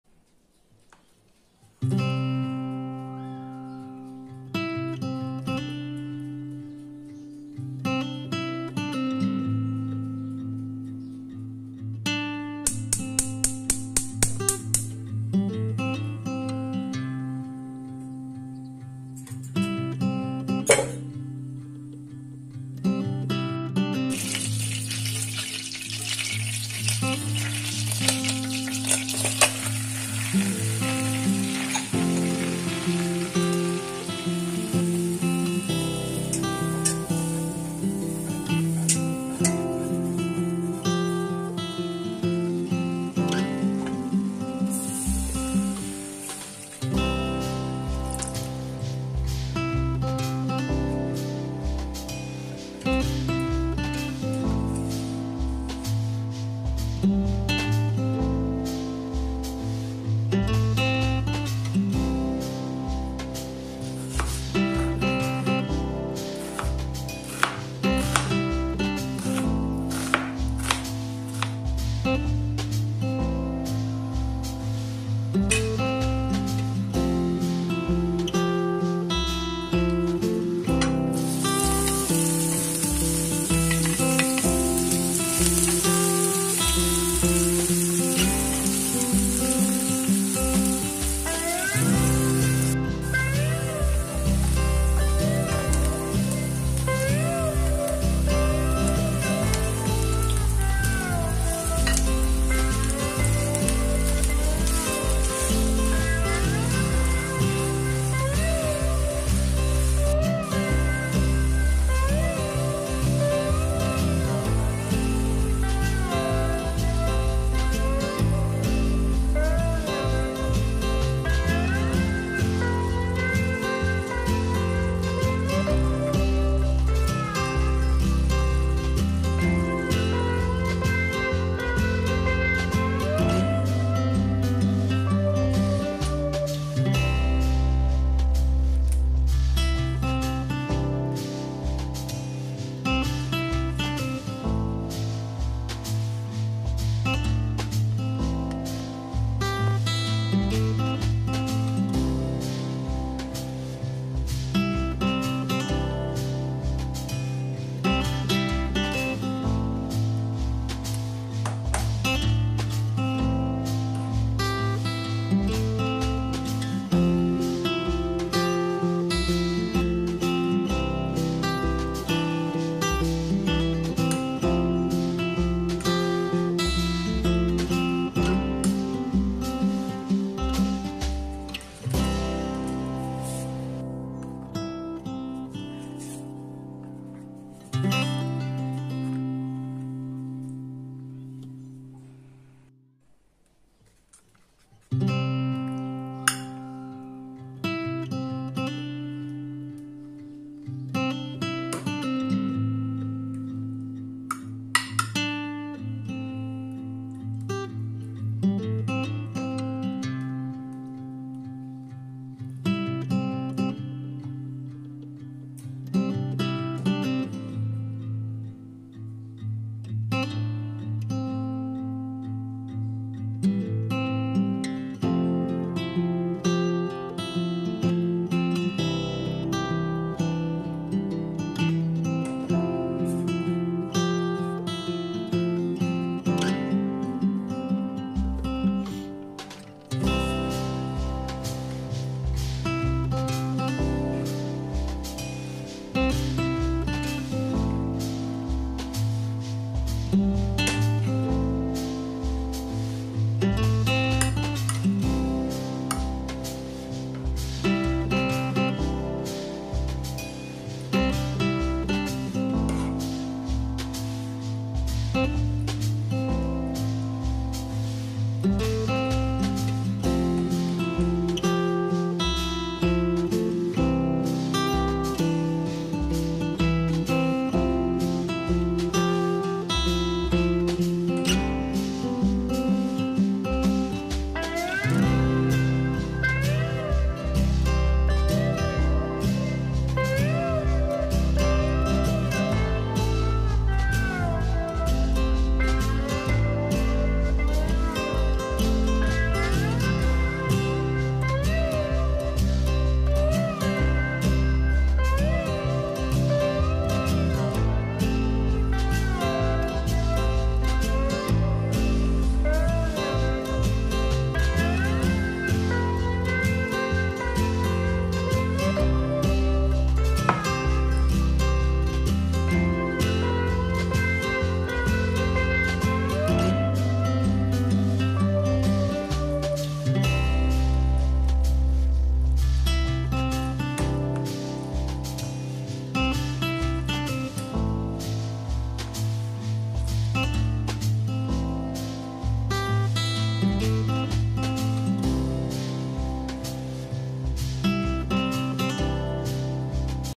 LASAÑA CASERA, FÁCIL Y RÁPIDO
Hoy preparamos esta deliciosa lasaña, muy fácil y rápida de hacer, con productos que todos tenemos en casa, lasaña casera preparada desde cero, delicioso 😋.